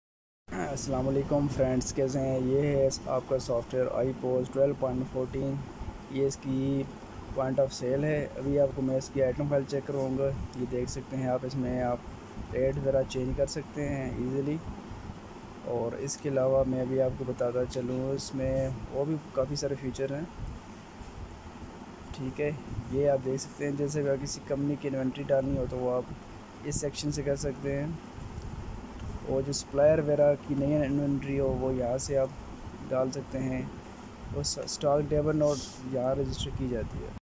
Ipos software 12.4 vlog 1
Ipos tutorial video basic points we learn all features like point of sales inventory control management systems and daily closing sytem like end of the day and many more su keeping support over software channel like and subscribe plz and bell icon press krna na bhole shukria